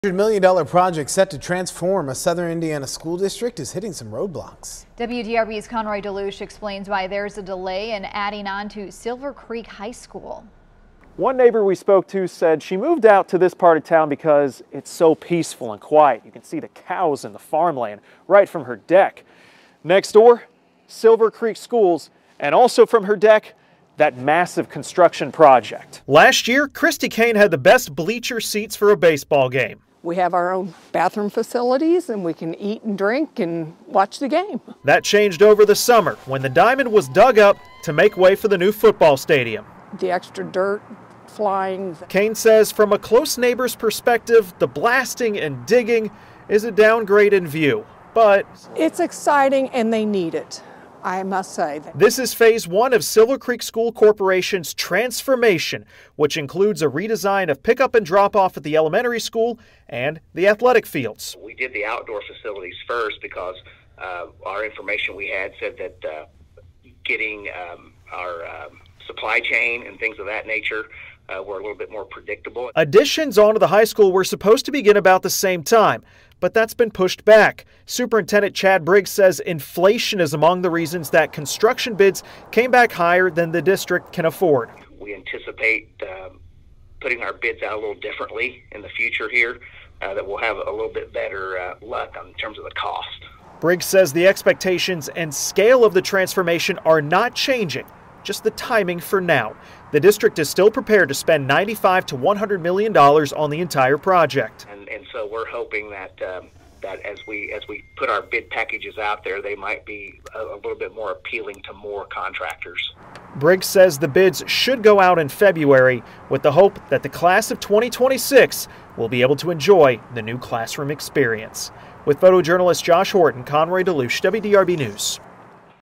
Rising costs causing roadblocks for $100 million renovation, expansion at Silver Creek Schools
Construction on the new athletic facilities at Silver Creek School Corporation's campus is already underway.